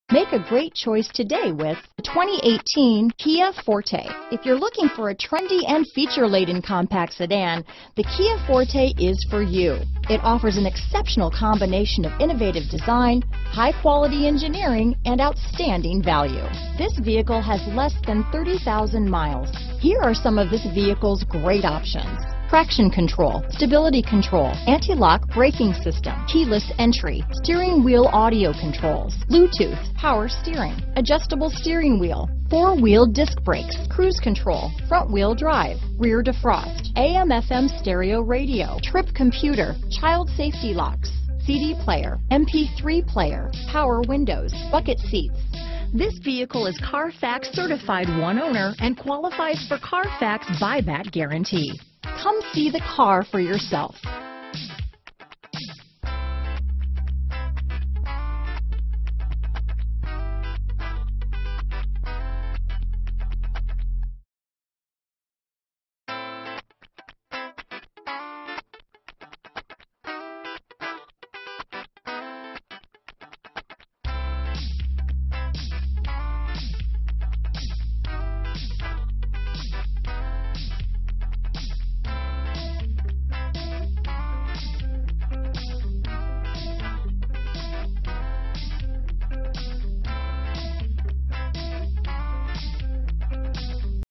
2018 Kia Forte Albany, Schenectady, Colonie, Troy, Glens Falls, NY 22001122
Aurora Black Used 2018 Kia Forte available in Albany, New York at Destination Kia. Servicing the Schenectady, Colonie, Troy, Glens Falls, NY area.

Destination Kia
760 Central Ave